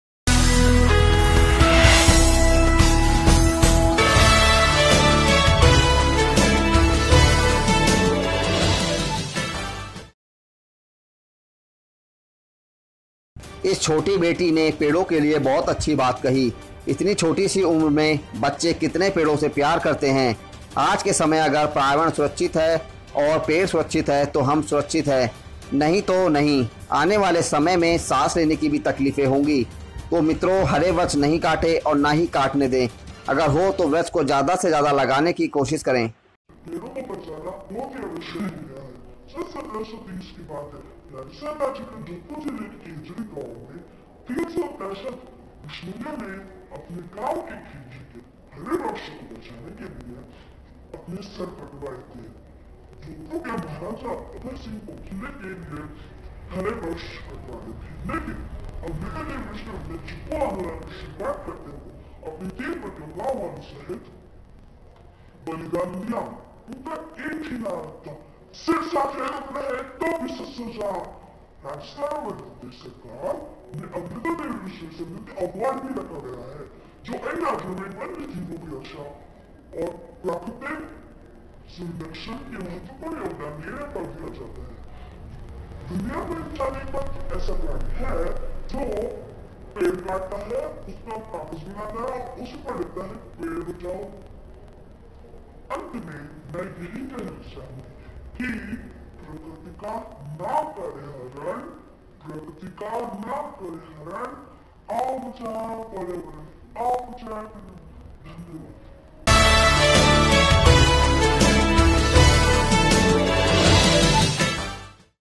इतनी छोटी सी उम्र में बच्चे कितने पेड़ों से प्यार करते हैं ।-Today24
इस छोटी बेटी ने पेड़ों के लिए बहुत अच्छी बात कही हैं इतनी छोटी सी उम्र में बच्चे कितने पेड़ों से प्यार करते हैं ।
आज के समय अगर पर्यावरण सुरक्षित हैं और पेड़ सुरक्षित हैं तो हम सुरक्षित हैं नहीं तो नहीं, आने वाले समय में सांस लेने की भी तकलीफ होगी ।
तो मित्रो हरे वृक्ष नहीं काटे और ना किसी को काटने दे, अगर हो तो वृक्ष ज्यादा से ज्यादा लगाने की कोशिश करें ।

T.V. News

Application.

views!!!

News."